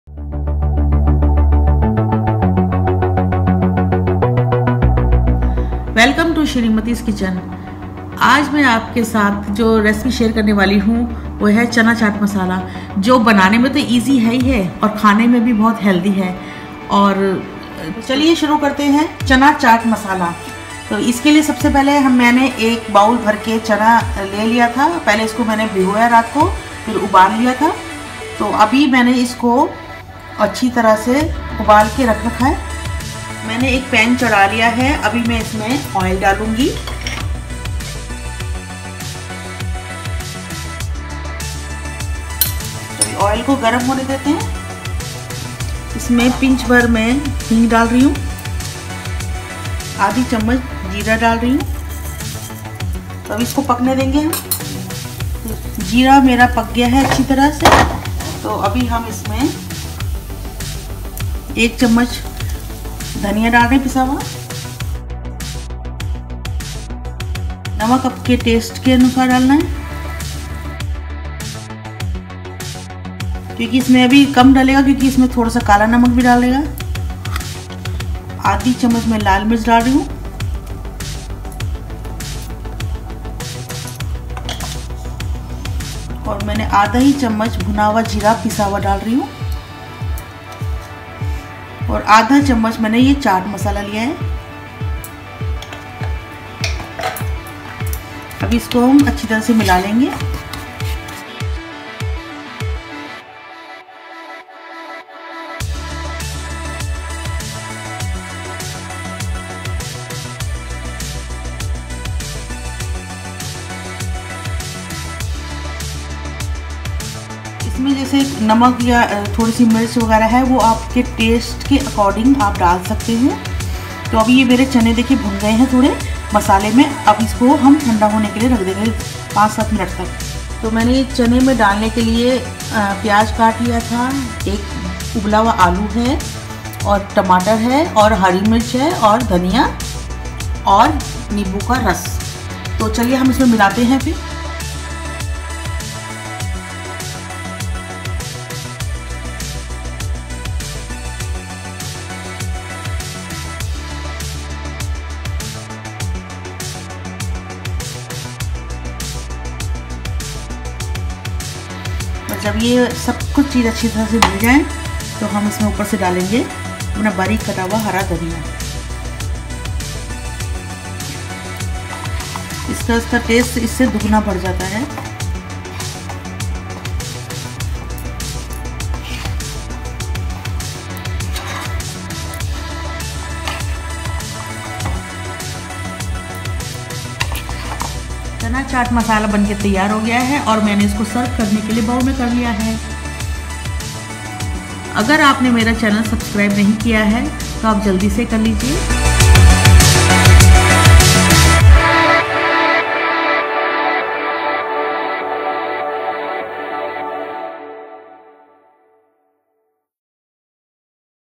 Chana chaat masala | winter fun | Shreemati’s kitchen 👩🏻‍🍳
Let’s have a little fun in the winters. enjoy a bowl full of protein in an amazing way. Let your stomach growl for it .

Ingredients required-:
Chana
Coriander leaves
Potato 🥔
Onion 🧅
Tomatoes 🍅
Lemon juice
Chilli 🌶
Salt 🧂
Dhaniya powder
Jeera
Chaat masala

Thas all. enjoy cooking. 😊.